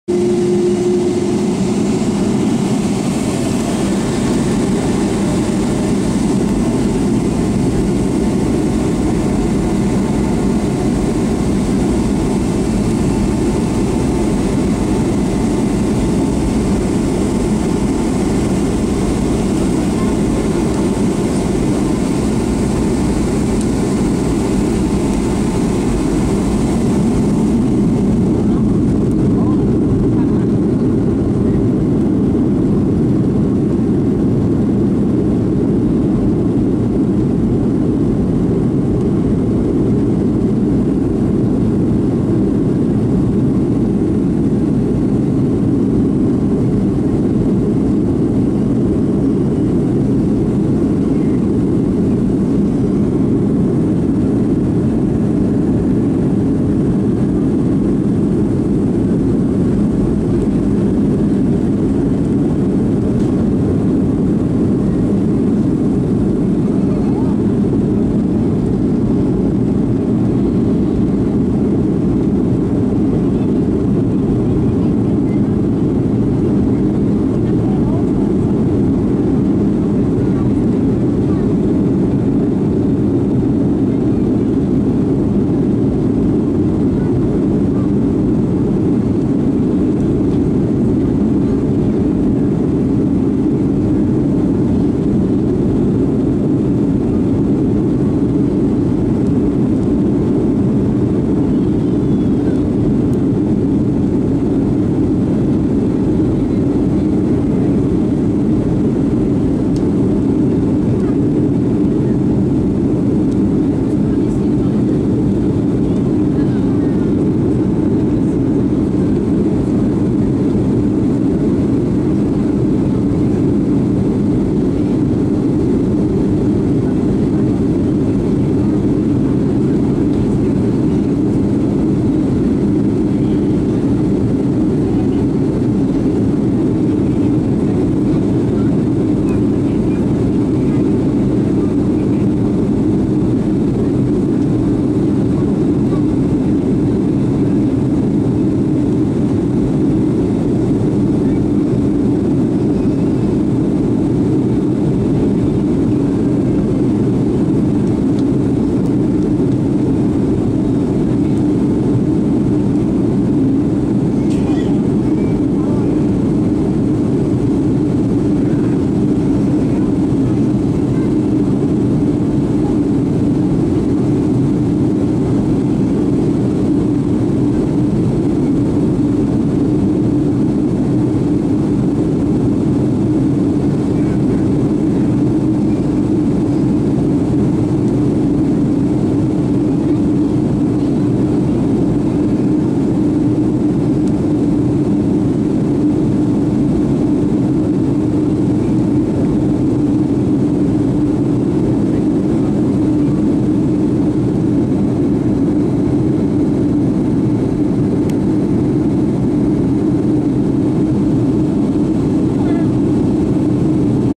Air Finland Boeing 757-200 OH-AFI takeoff from Helsinki-Vantaa Airport
Video filmed on 15th june 2011 on Air Finland flight from Helsinki-Vantaa, Finland to Las Palmas, Spain. Airplane was Boeing 757-200 OH-AFI.